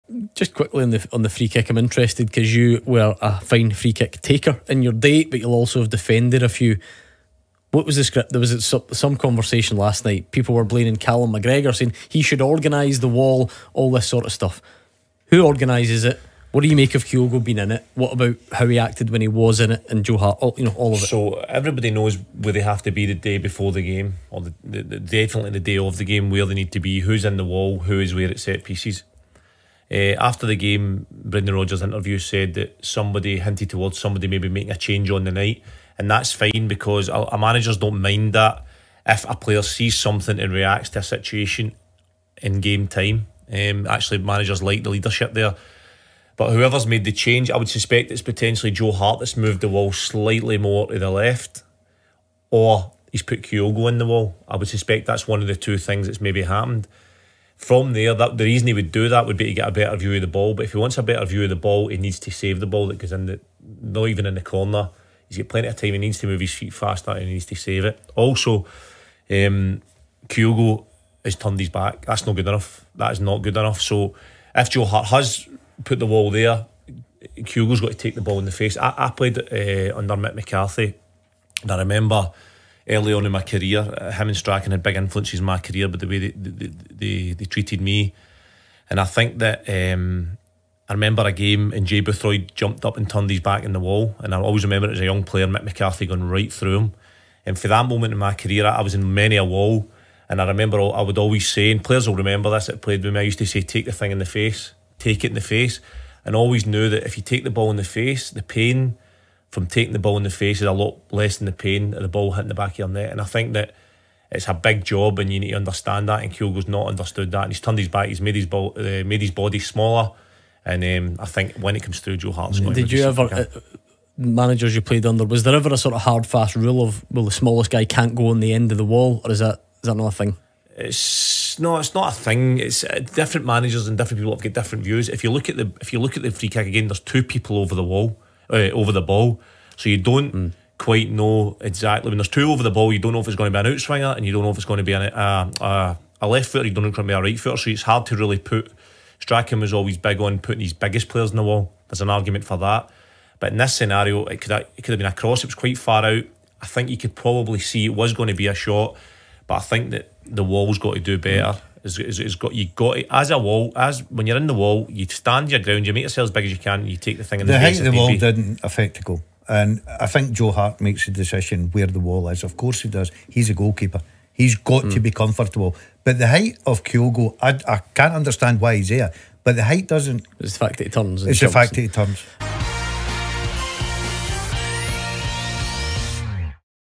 ⚽Charlie Mulgrew talks through who was at fault for Feyenoords freekick
Former Celtic defender Charlie Mulgrew talks through the free kick goal which his former team conceded against Feyenoord.

📞 Phone in: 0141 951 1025
🖥 Tweet @ClydeSSB